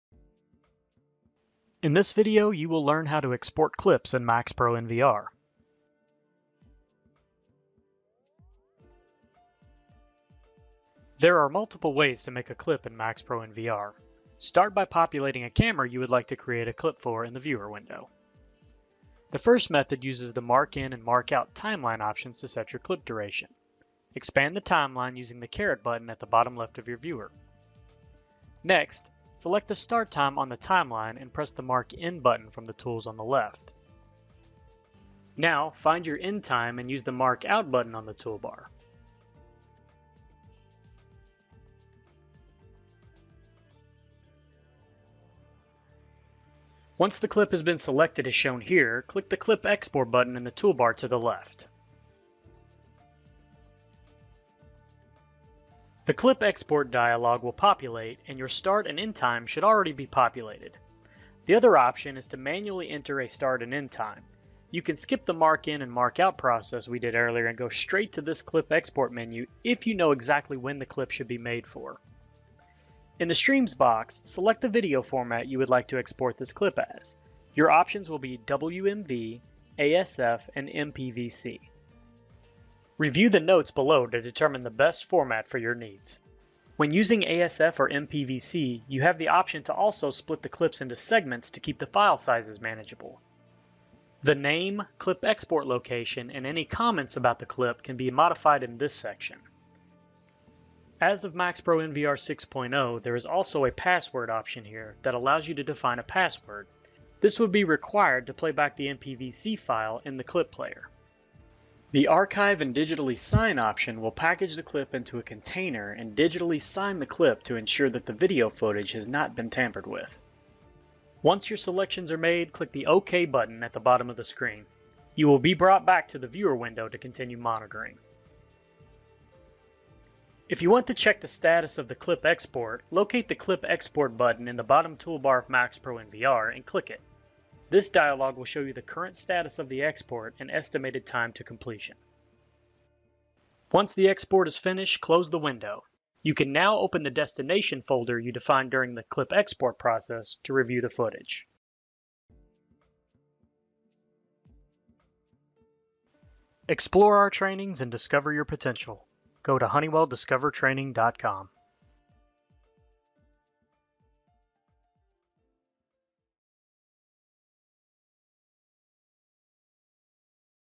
How To Save Clips in MAXPRO® NVR
In this video, you will learn how to save and export clips from MAXPRO® NVR.